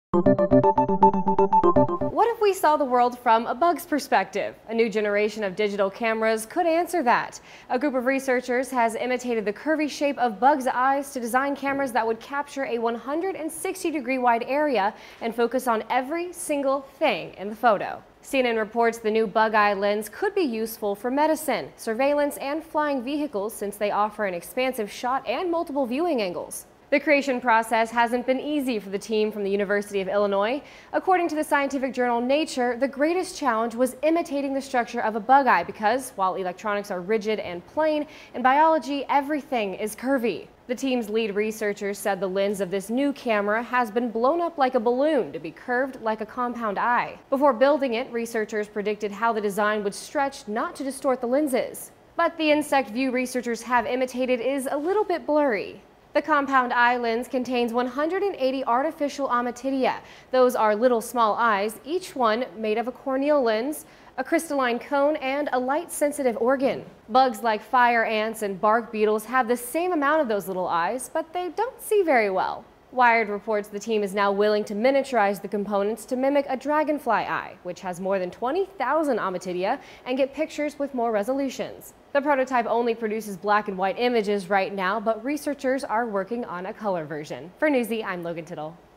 Digital Camera Gives Bug's-Eye View
Researchers hope the camera's practical applications will include medicine and surveillance.